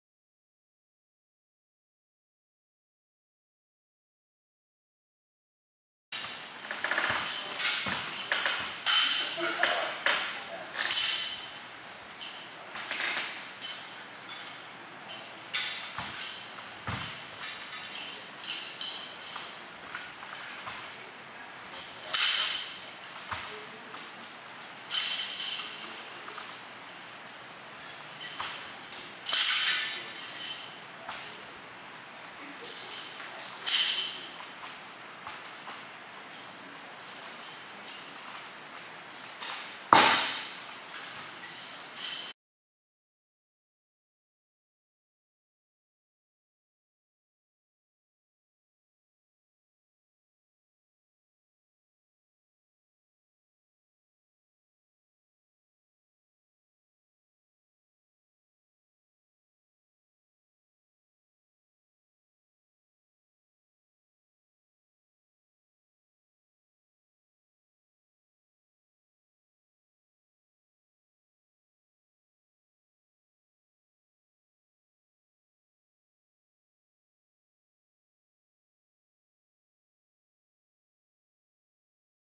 MG Archive Xiao Feng Kwan Dao Part 03
In this film, Master Xiao Feng continues the kwan dao form.